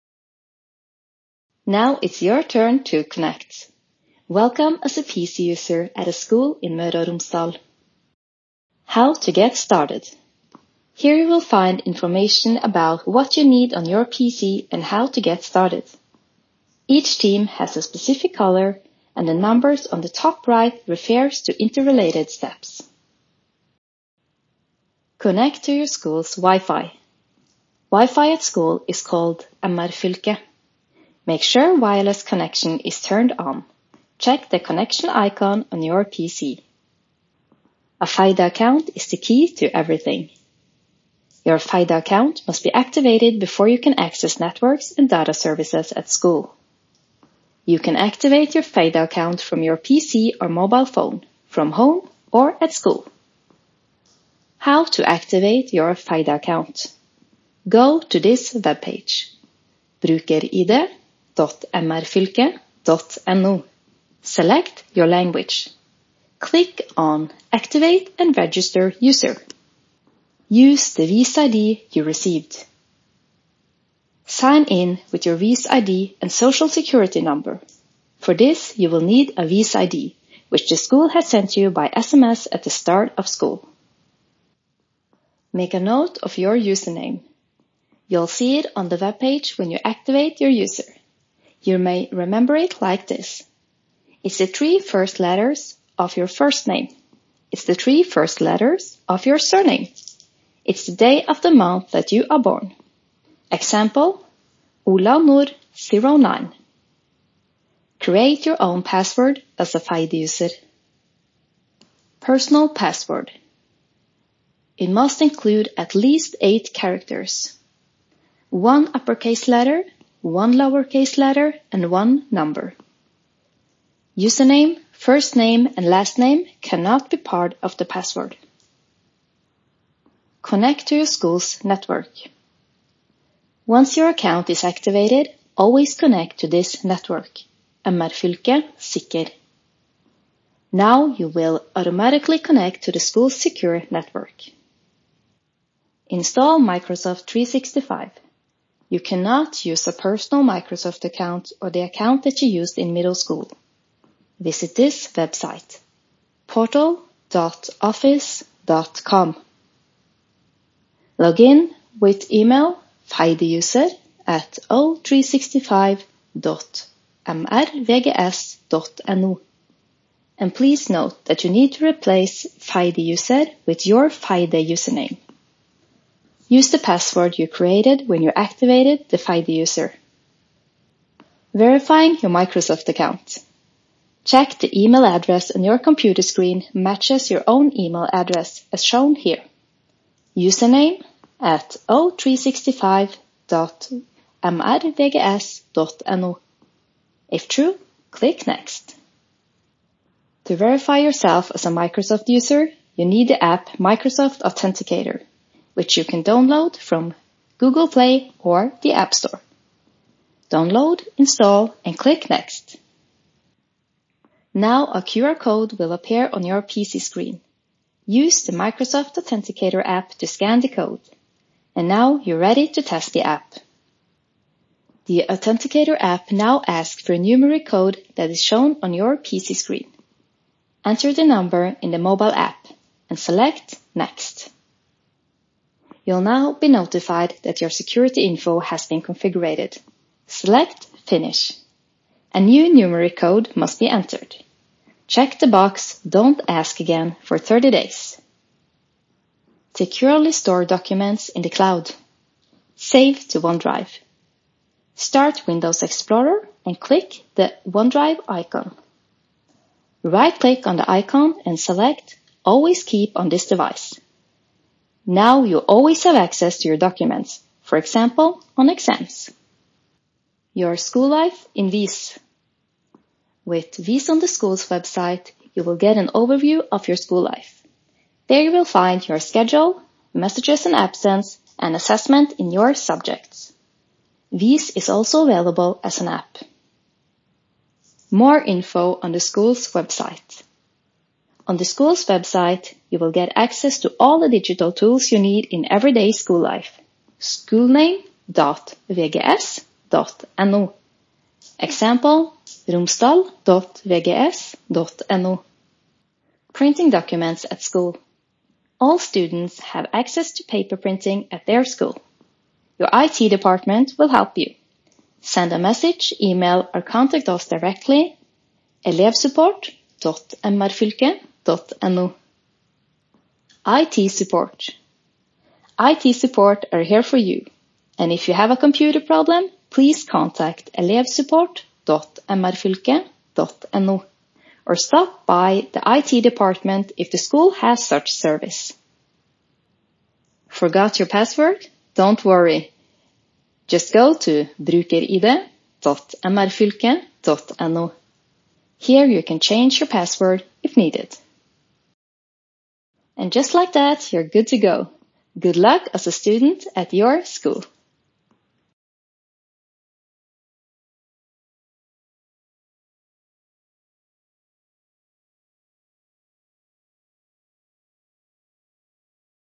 Start VGS Møre og Romsdal 2024 (English)
In this film, you get information about what you need on your PC and how to get started:
- connect to the school's network
- activate your Feide account
- install and verify Microsoft 365 account
- safe storage of documents
- what you can find in VIS
- printing documents
- IT support
- help if you forget your password